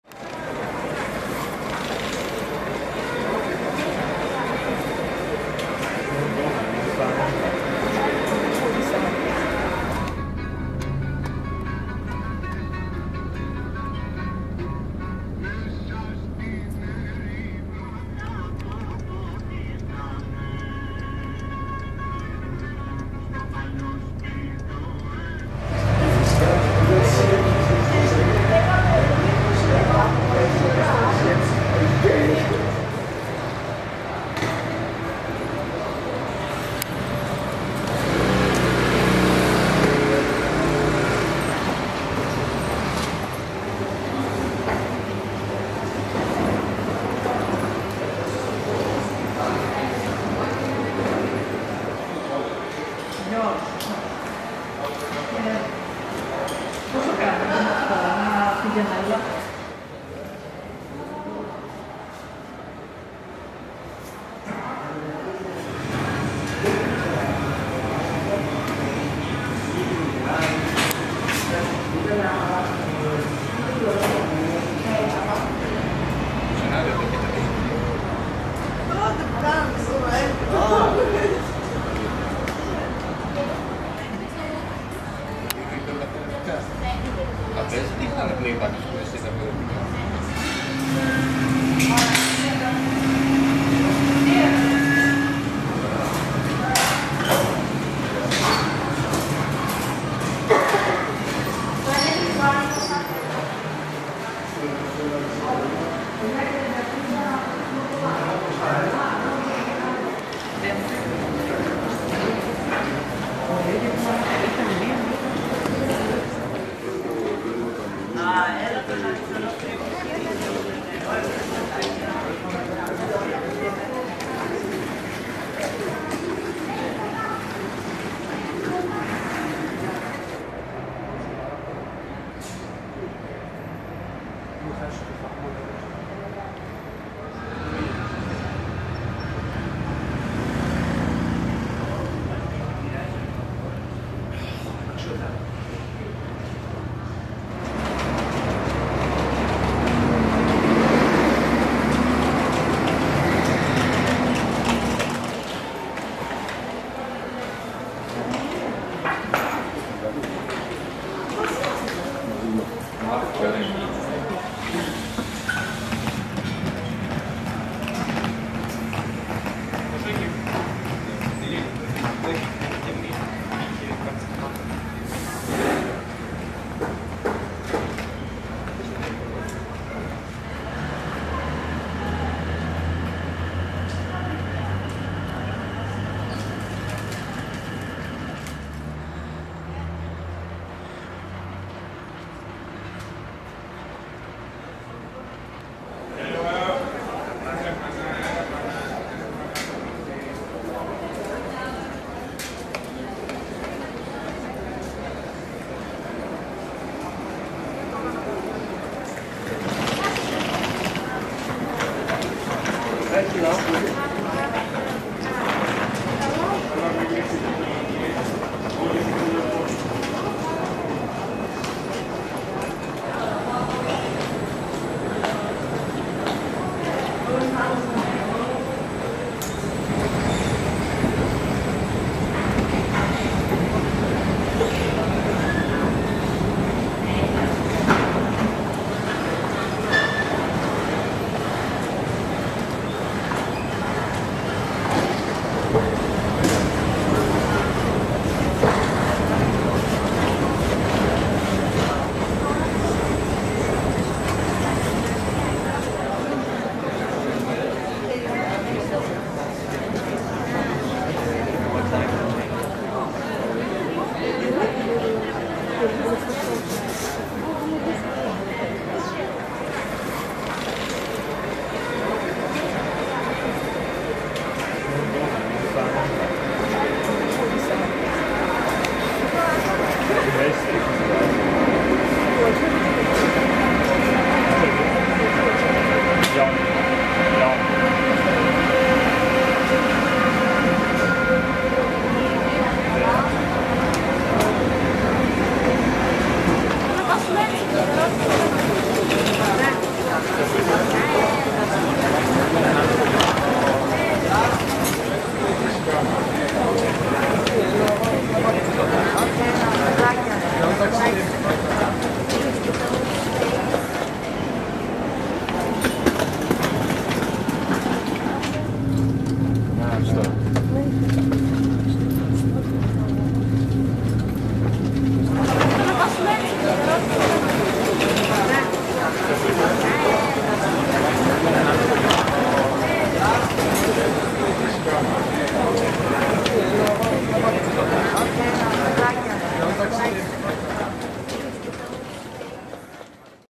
A Look At Athens Main Train Station, Athens, Greece
Athens railway station is the main railway station of Athens, and the second largest station in Greece. Located in the central quarter of Kolonos, it resulted from the merger of the city's two main railway terminals—the Larissa Station of the Piraeus–Platy railway towards central and northern Greece, and the Peloponnese Station of the Piraeus–Patras railway that formerly linked Athens with the Peloponnese. It is still colloquially known as Larissa Station, which is also the name of the adjacent Metro station.